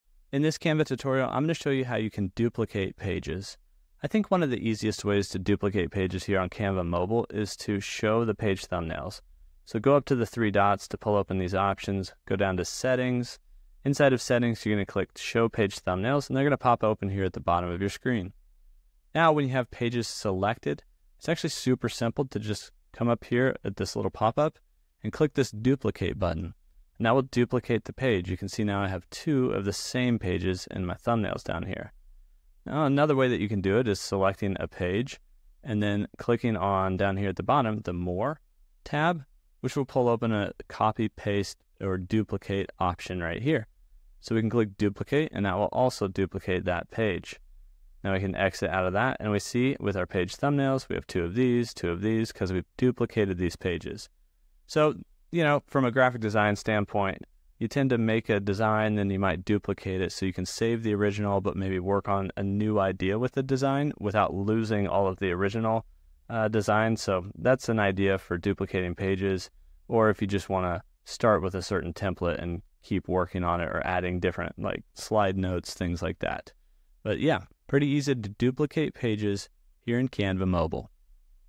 How to Duplicate Pages on Canva
In this Canva tutorial, learn how to duplicate pages on Canva.